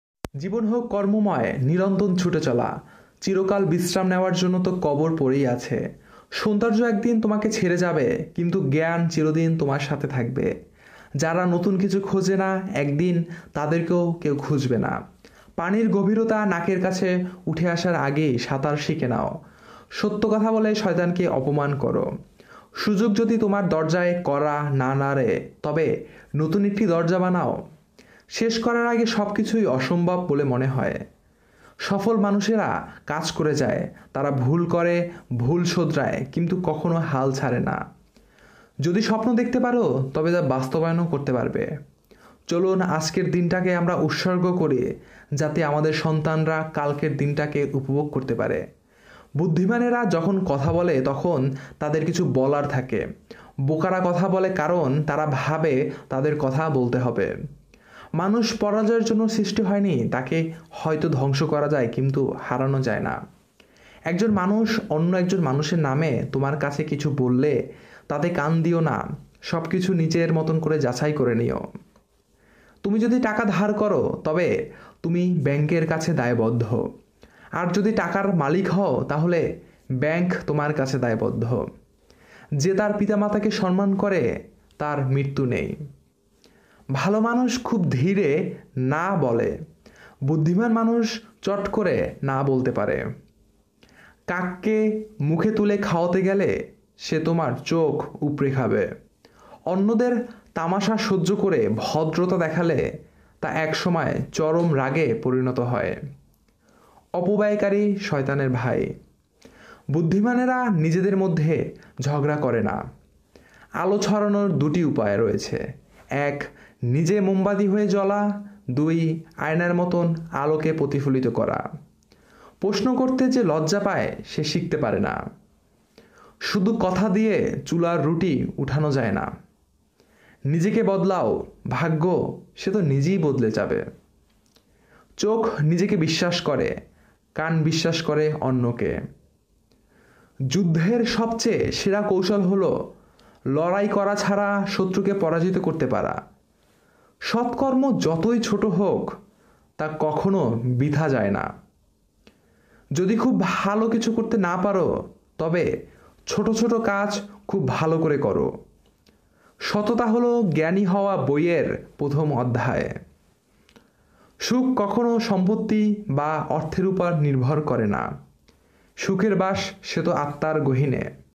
৩০টি বিখ্যাত উক্তি যা আপনাকে সব সময়ে অনুপ্রেরণা দিবে | Bangla Preneur | bangla new quotes 2021
৩০টি বিখ্যাত উক্তি যা আপনাকে সব সময়ে অনুপ্রেরণা দিবে। জীবন সম্পর্কিত উক্তি যা আপনার চিন্তাভাবনা পরিবর্তন করতে সাহায্য করবে।

মোটিভেশনাল উক্তি
শিক্ষামূলক উক্তি
মনীষীদের উক্তি
প্রেরণামূলক উক্তি
একতা নিয়ে উক্তি
মুসলিম মনীষীদের বিখ্যাত উক্তি
বিশ্বাস নিয়ে উক্তি